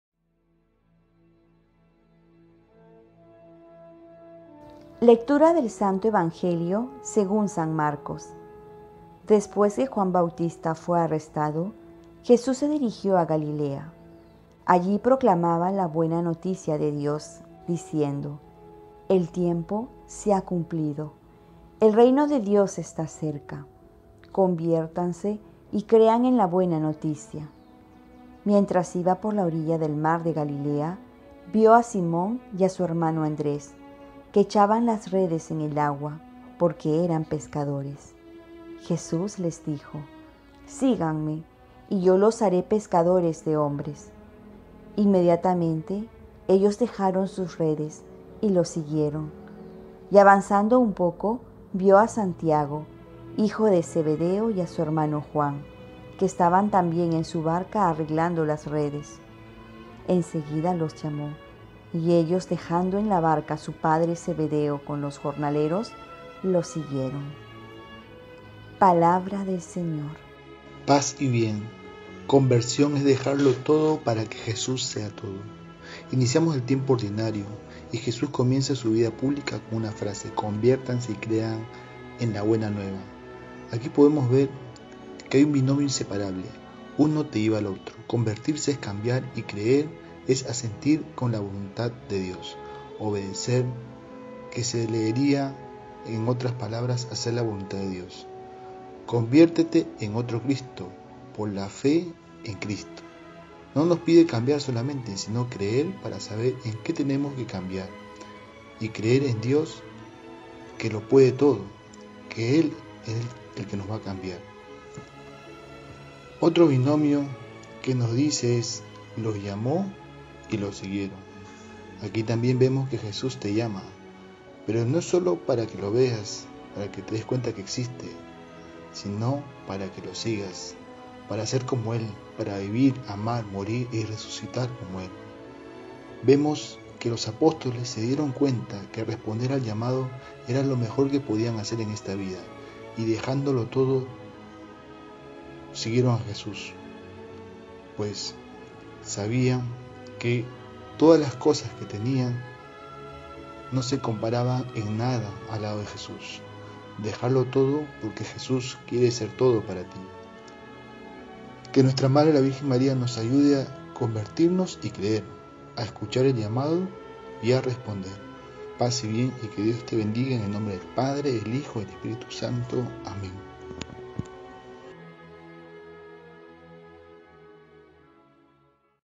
Evangelio del lunes 13 de enero del 2020 Mc 1,14-20
Breve reflexión del evangelio por el Hno David Pacheco Neyra OFM Cap.